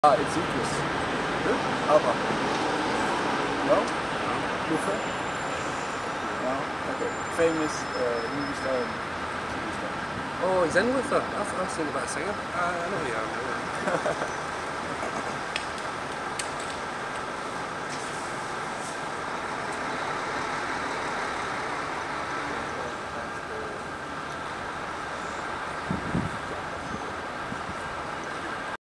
Idris Elba Filming Series 4 Of Luther In London
Actor Idris Elba is seen filming series 4 of the Hit award winning TV Show  'Luther' with actress Laura Haddock in London, England.